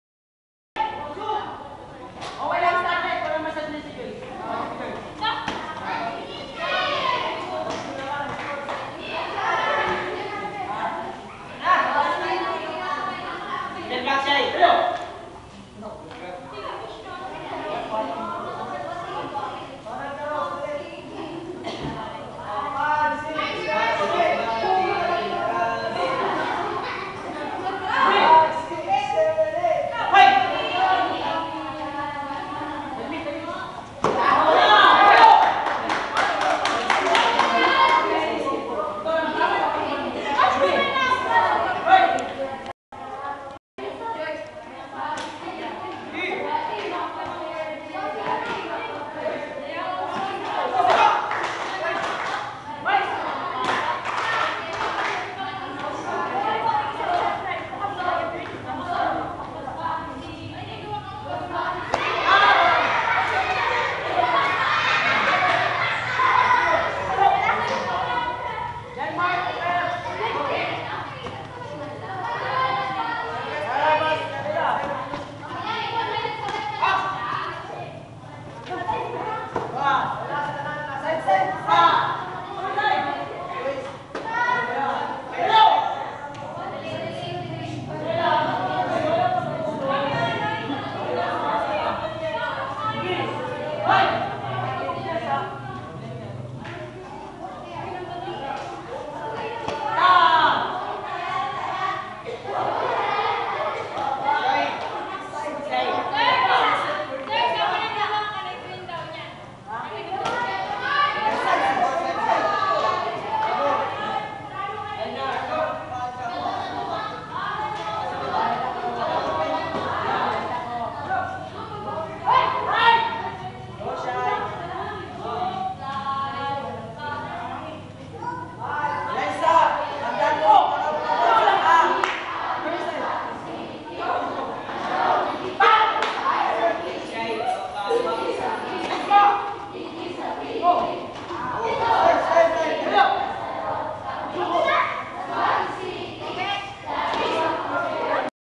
PB2015,Training, March 7, 2015, MNCHS Student Center
Ma. Shayla Krisna Espinosa (Red Armor) vs Joyce Patay (Blue Armor)